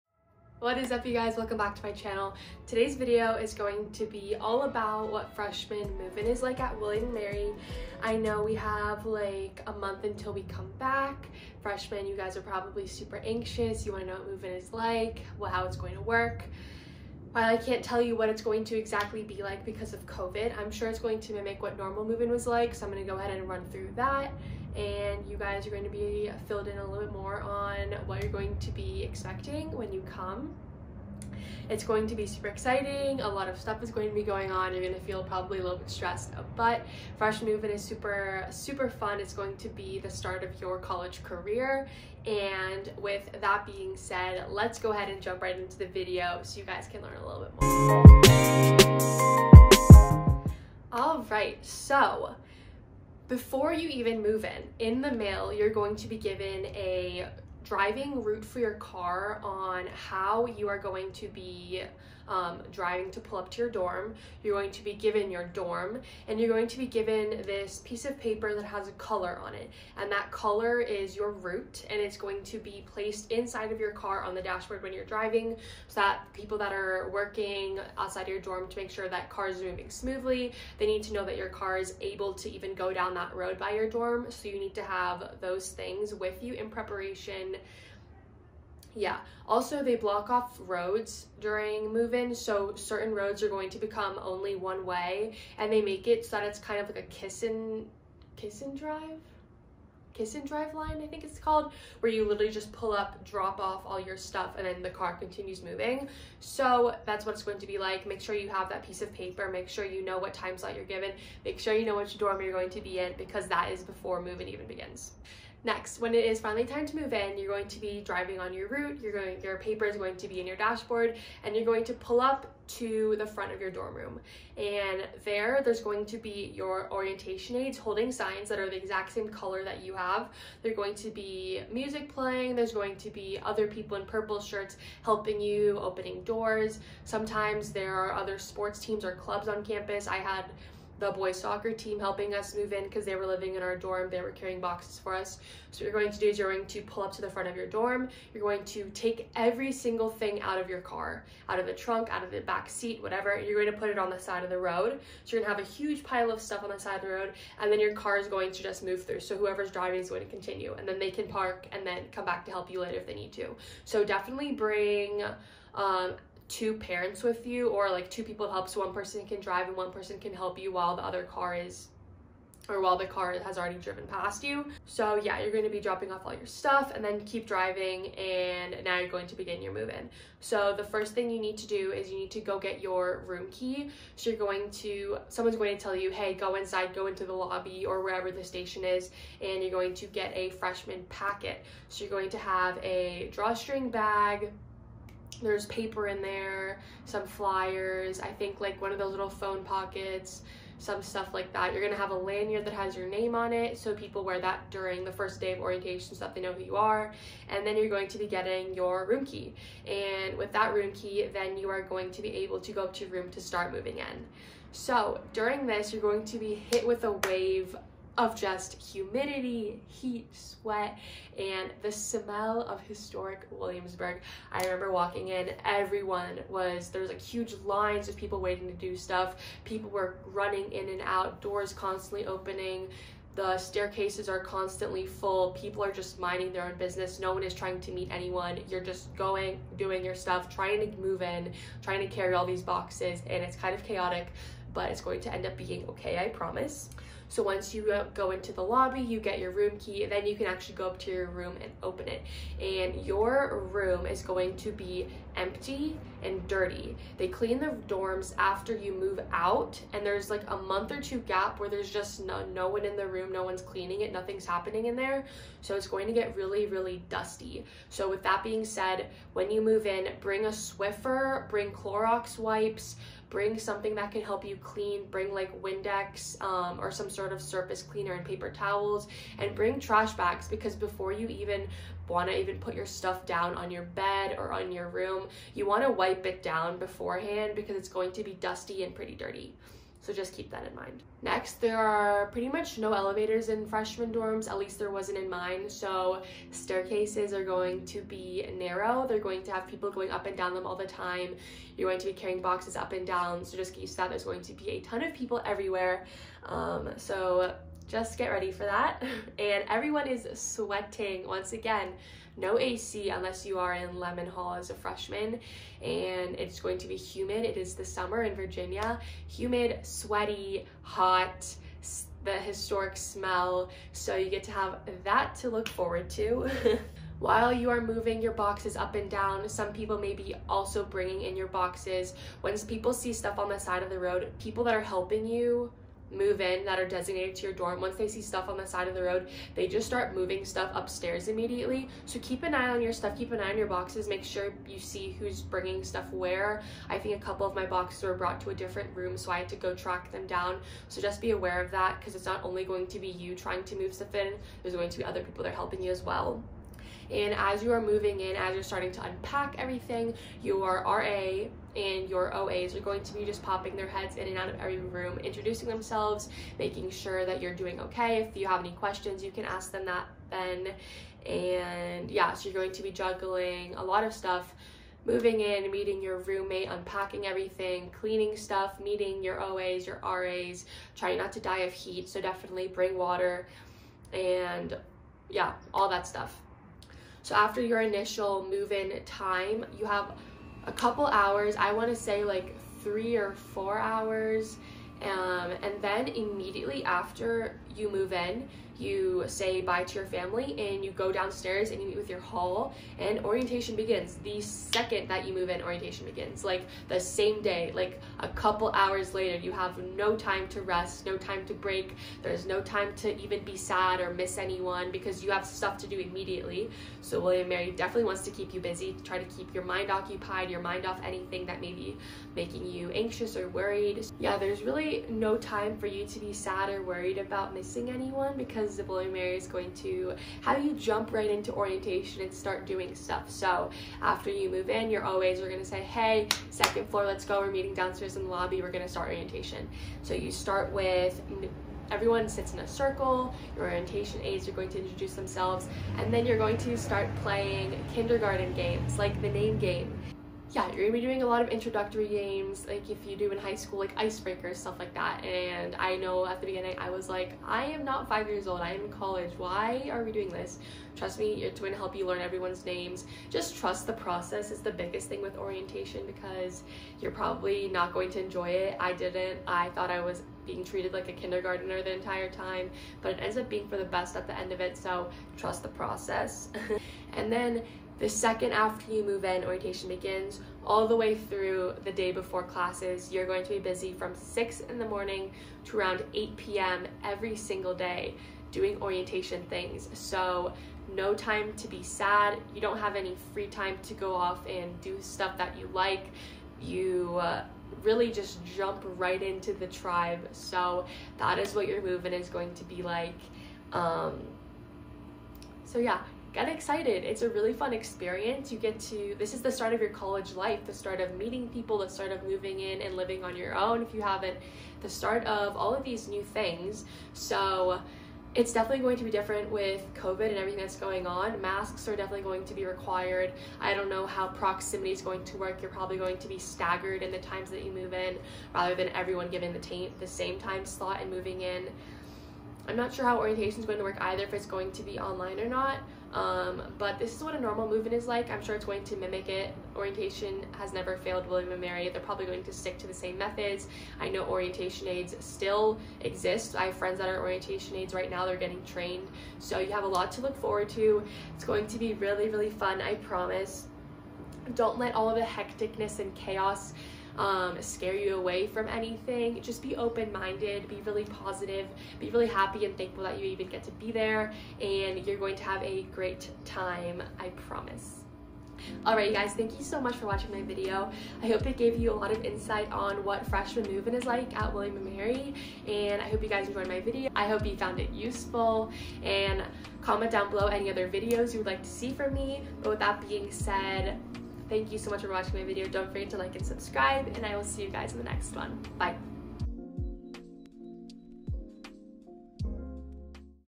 FRESHMAN MOVE-IN AT WILLIAM AND MARY || What to expect, Tips +Tricks, & More!
Hey guys!
I hope it helped you guys with what to expect for move in. While I can't exactly describe how it will work due to covid, this is what normal move in is like and it should mimic it with a few modifications!
Background Music!
Favorites:
a b o u t m e:
-Gabi Burgin
-19
-Sophomore at The College of William and Mary
-Healthy lifestyle & wellness posts
-DMV x LVGS
Have an amazing day,
xoxo gbf